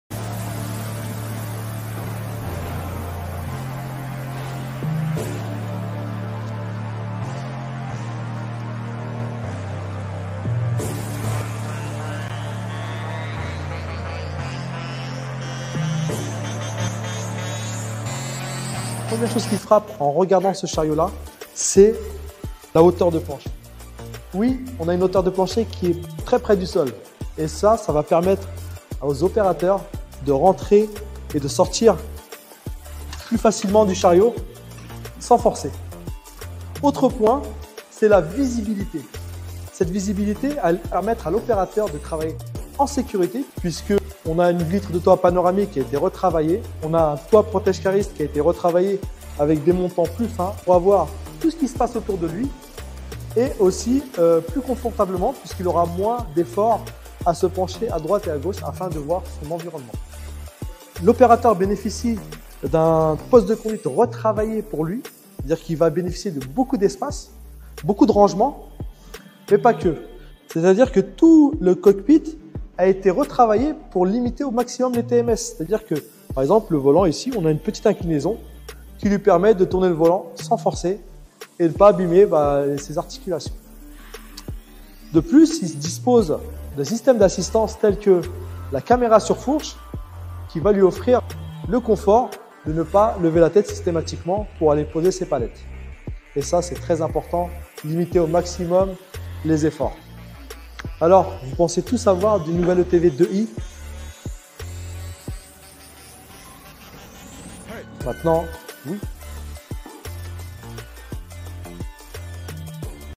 Nouvel ETV2i - Découvrez ses améliorations ergonomiques
🎥 Notre Chef Produit, Mourad, vous dévoile enfin tous les détails du nouveau chariot à mât rétractable ETV 2i !

🔍 Améliorations ergonomiques, confort d’utilisation et puissance : découvrez dans cette vidéo comment l'ETV 2i est conçu pour des performances maximales au quotidien.

🚀 Boostez votre productivité avec cette nouvelle gamme pensée pour vos besoins.